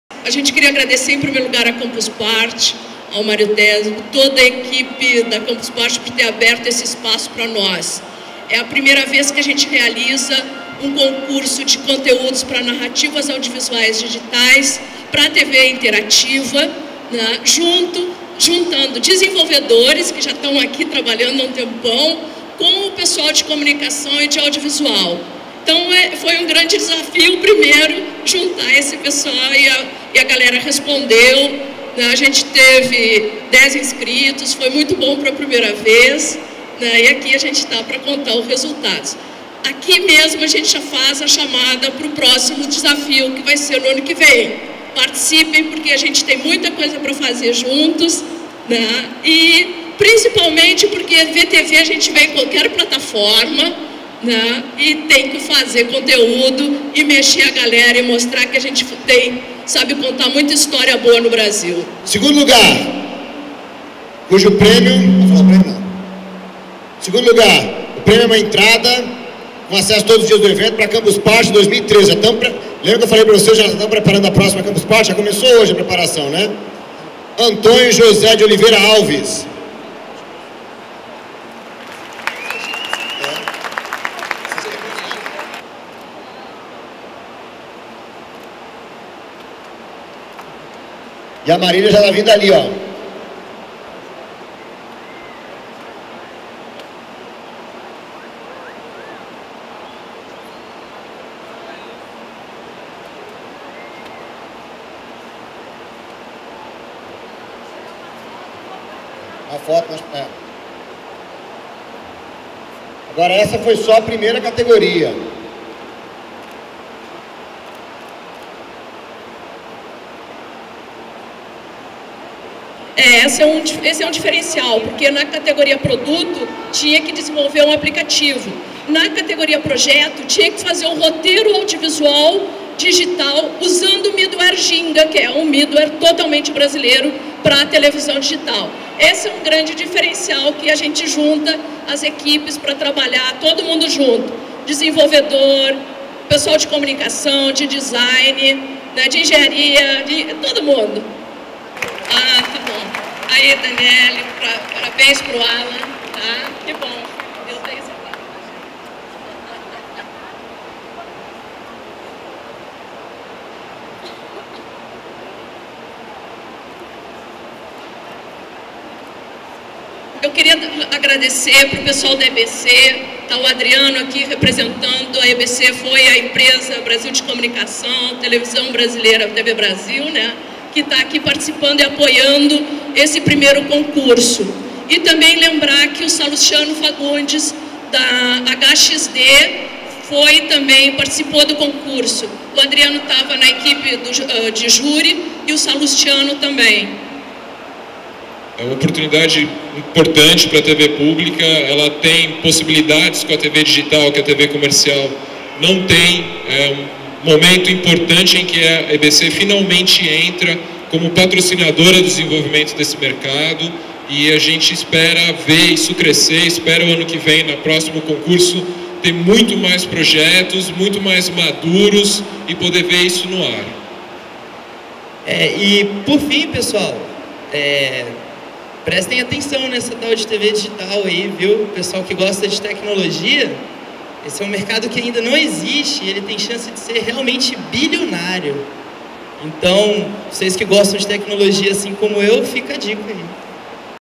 Premiação dos melhores aplicativos para TV Digital na Campus Party
Pela primeira vez na história da Campus Party, foram premiados desenvolvedores de aplicativos e projetos de narrativa multimídia para rodar no middleware Ginga.

O prêmio foi apoiado pela Empresa Brasil de Comunicação - EBC.